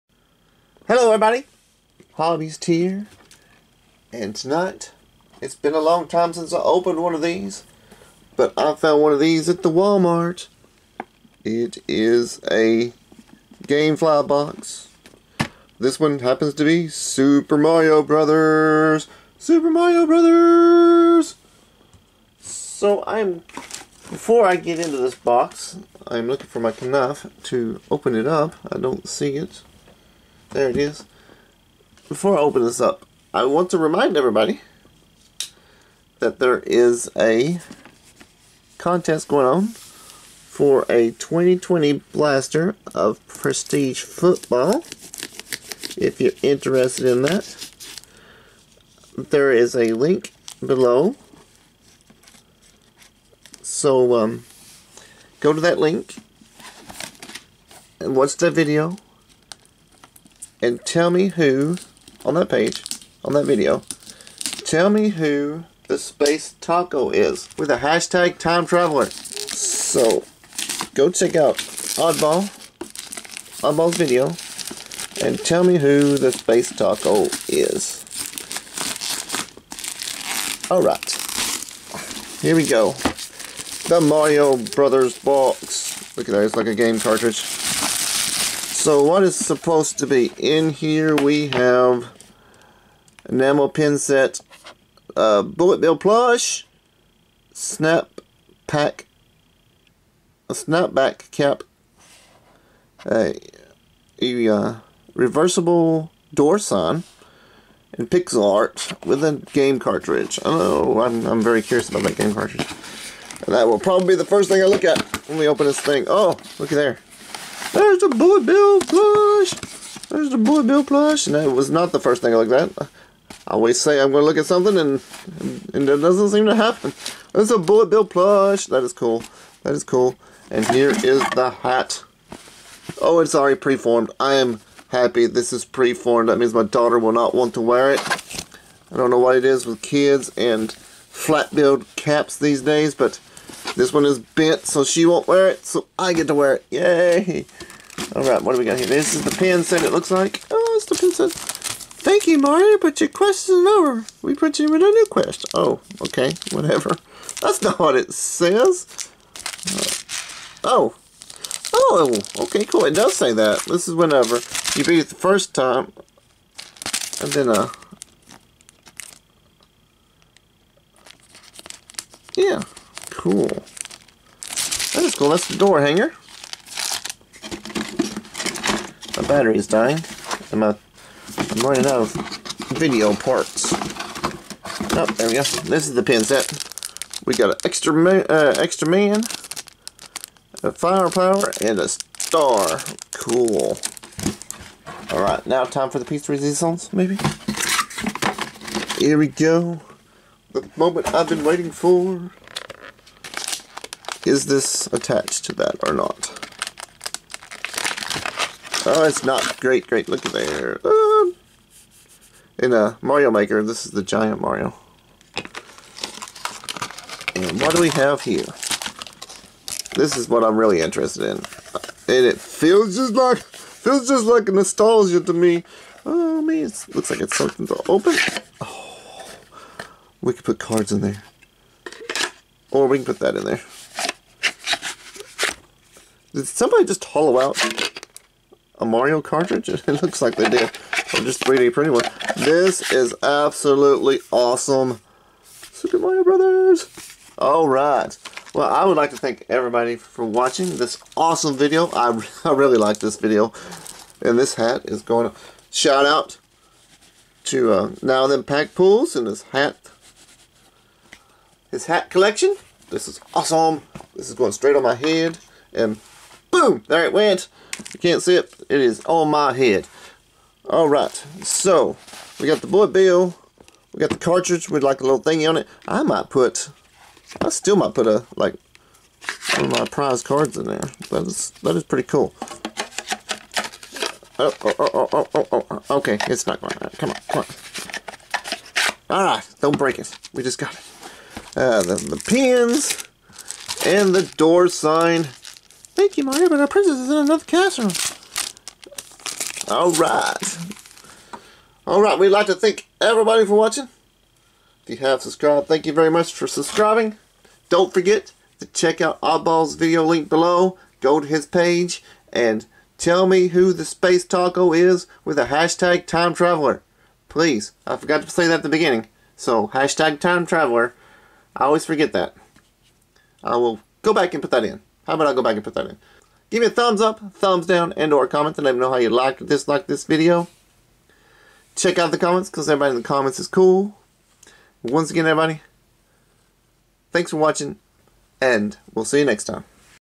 Loot Box: Episode 5
It's sure has been a while!  How about opening a Super Mario Bros. loot box?  This is going to be fun!